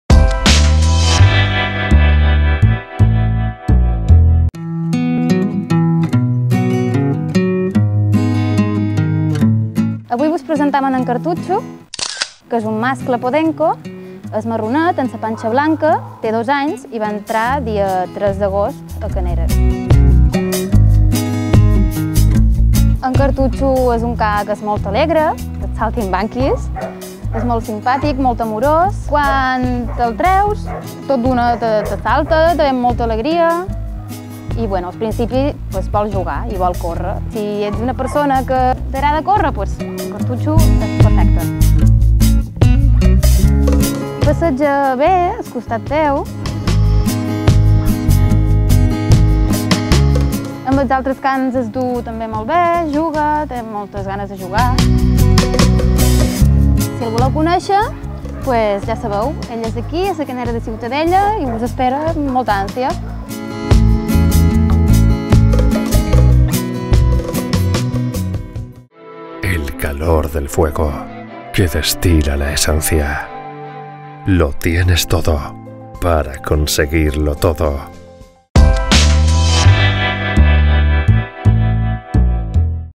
2018 09 28 MenorCa Adopta Cartutxo Podenco
Avui el protagonista de MenorCa Adopta, es nomena Cartutxo, com comenta la Roser, voluntaria de la Protectora de Ciutadella, és un saltinbanquis, però, que polit que és.